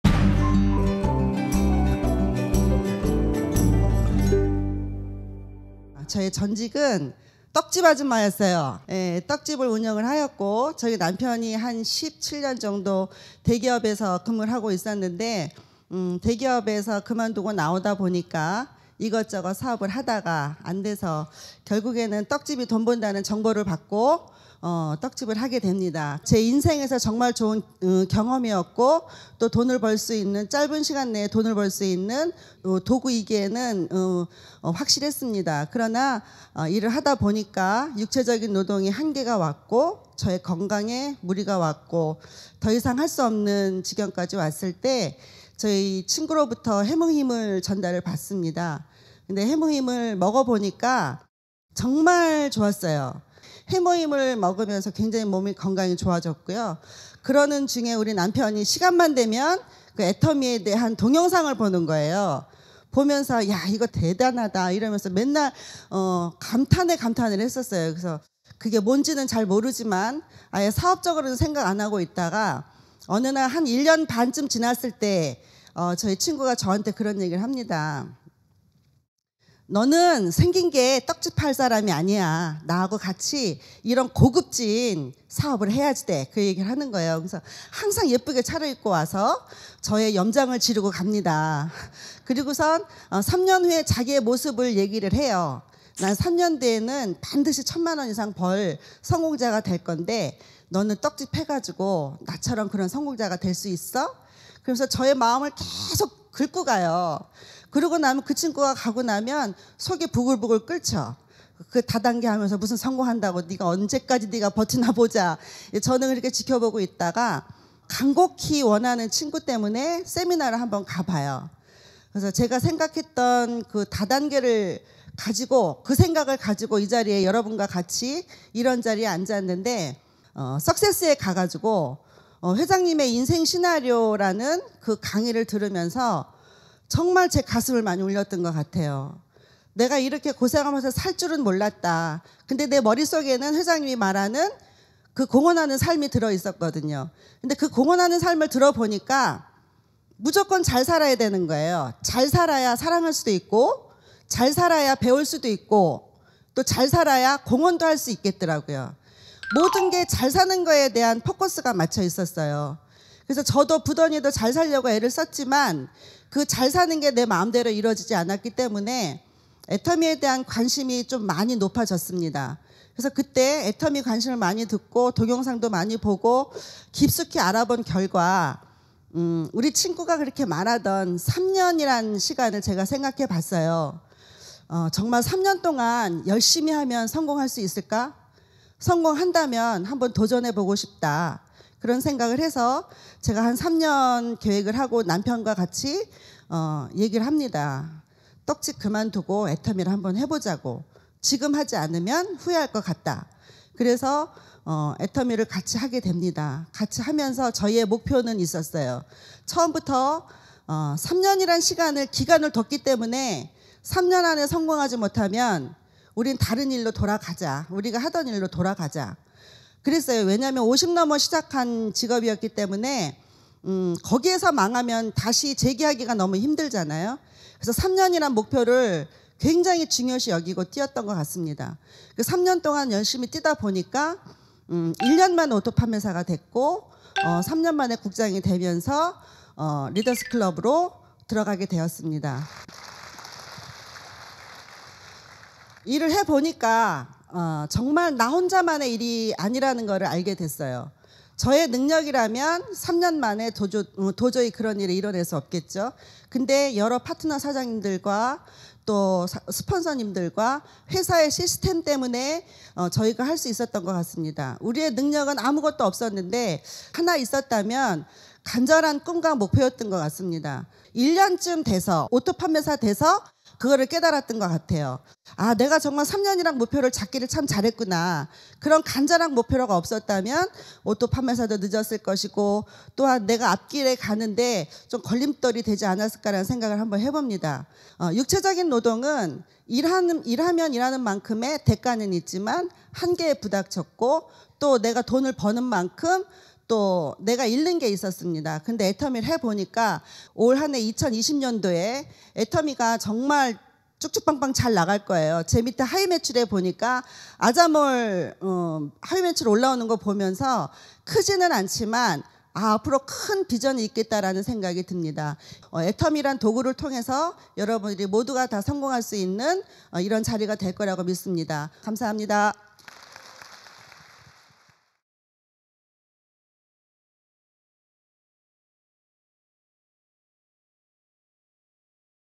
[리더환영사]잘 살아야 할 수 있는 것들ㅣ홍승연 STㅣ인천원데이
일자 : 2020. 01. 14
장소 : 인천 원데이 세미나

사랑하고 배우고 공헌을 하기 위해 필요한 건
잘 사는 것!

잘 살기 위해 목표를 가지고 달렸습니다

능력은 없었지만 혼자가 아니었기에 가능했다고 말하는
홍승연 스타마스터,

누구나 할 수 있습니다
지금 당장 목표를 세우고 나아가면 됩니다

영혼을 소중히 여기는 기업 애터미(주) 공식  YouTube채널입니다.
애터미를 사랑해주시는 모든 분들을 위한 채널입니다!